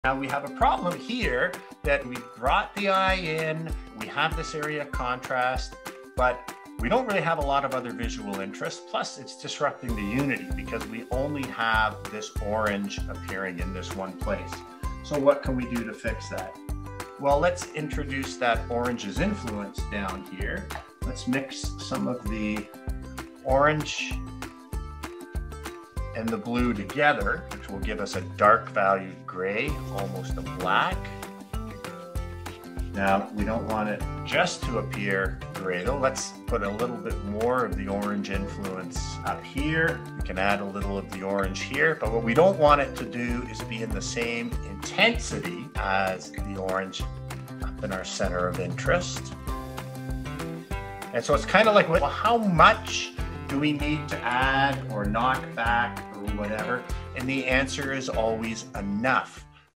Maintaining Unity
We need to echo the influence of the centre of interest around the painting in order to preserve the painting's unity🎨